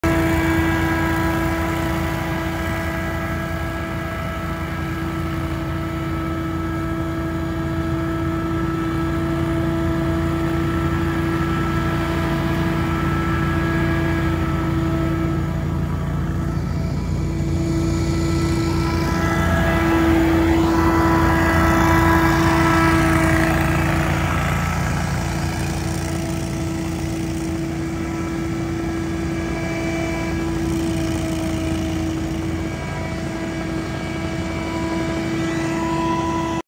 ** SCAG WINDSTORM IN ACTION IN HEAVY FALL LEAVES ***
SCAG WINDSTORM IN ACTION IN SOME HEAVY FALL LEAVES!